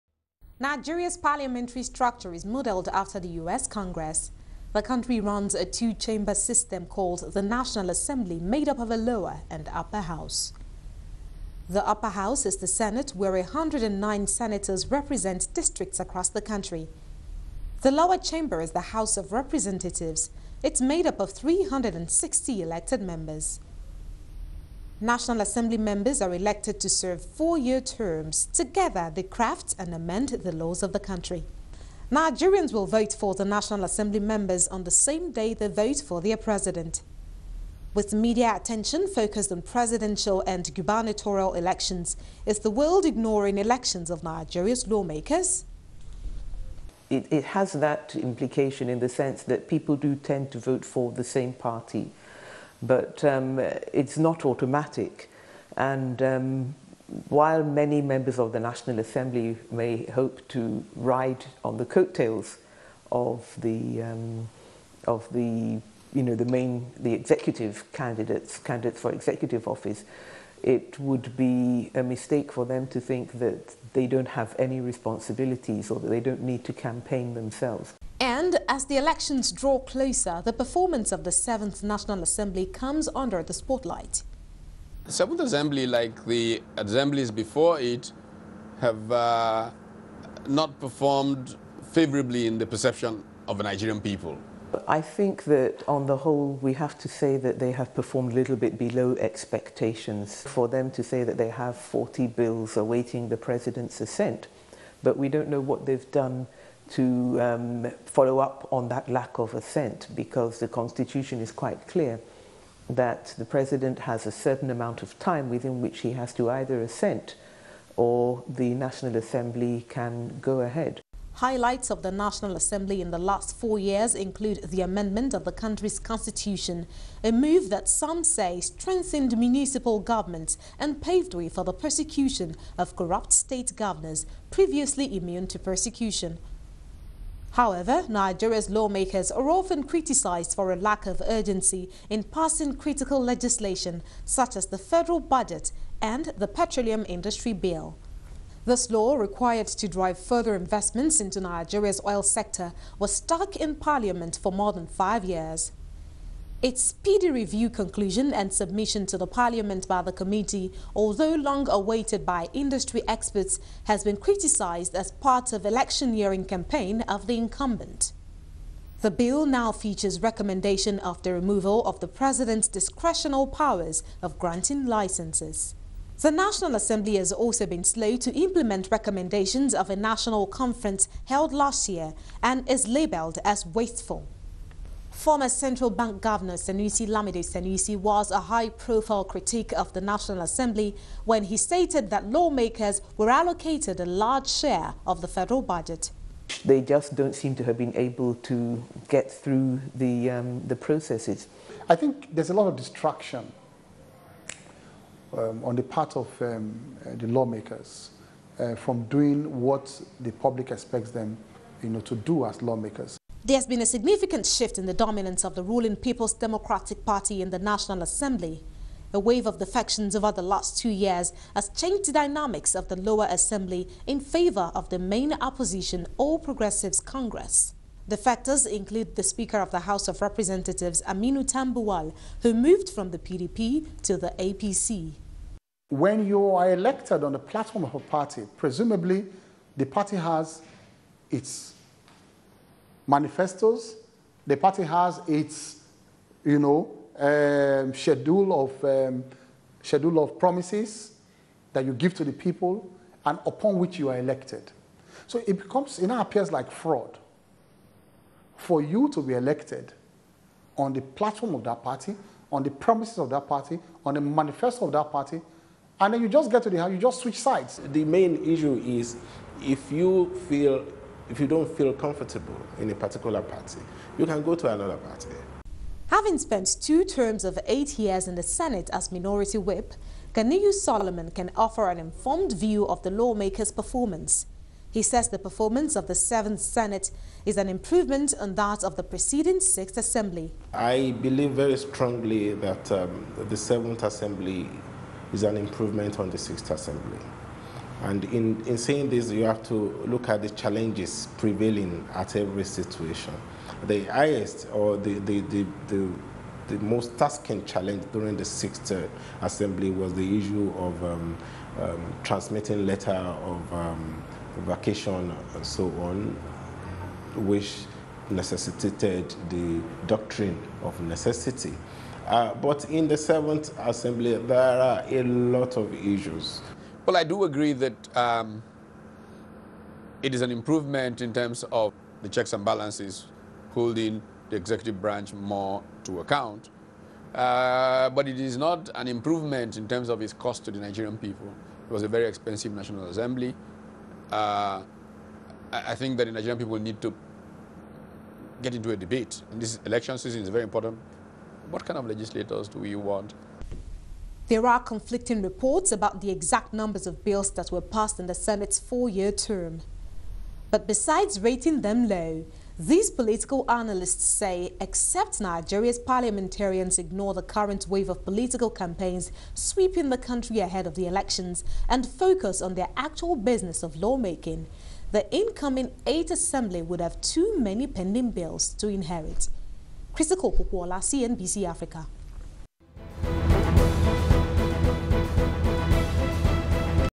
How Nigeria's parliament works
CNBC Africa's Christycole Popoola examines  how Nigeria's parliamentary system works and how crucial this is to its 2015 elections.